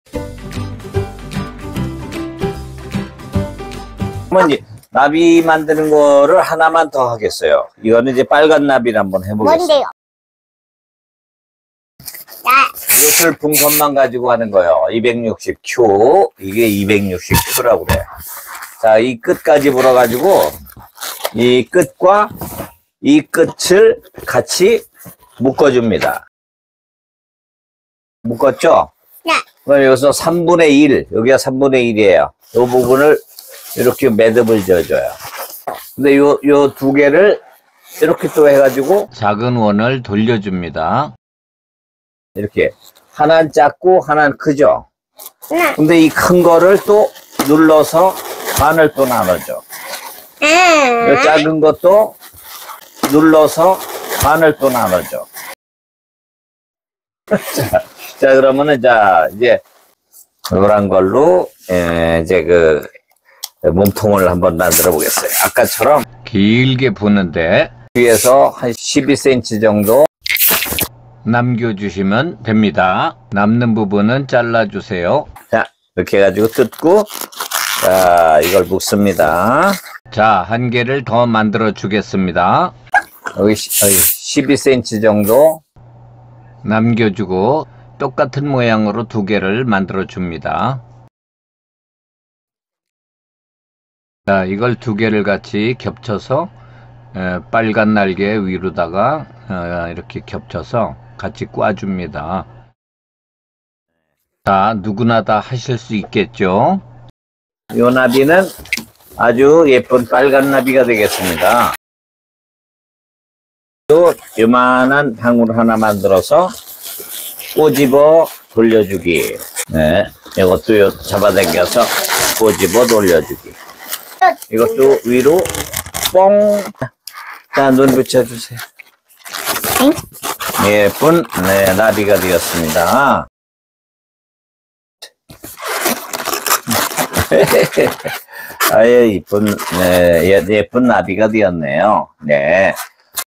(하은이와 함께)나비 만들기(2)
260Q풍선만으로 나비 만드는 방법을 가르칩니다.